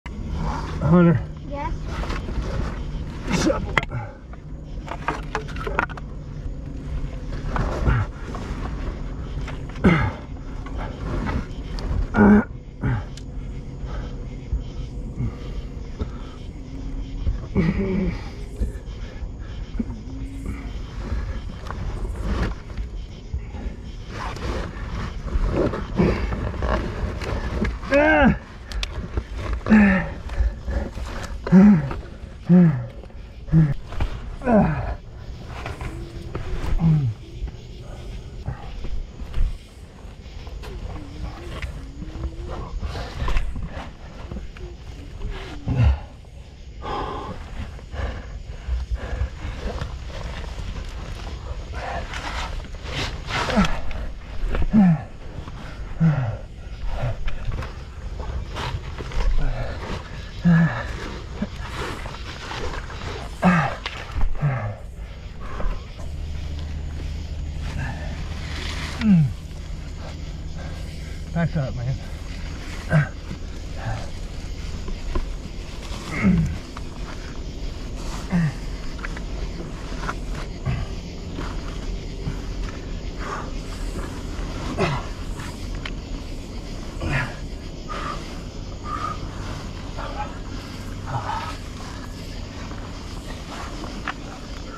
Very tight exit hole! It was a tough pull!
It was all I could to to pull myself up and out of this narrow exit hole. We just dug out this new cave entrance and I really should dig out some more because I was almost pinched off inside! Thankfully I had a chain in place to help me pull myself up out of the hole!

 *Warning!
 I am a professional caver in a controlled environment.
Never enter a cave without a cave guide!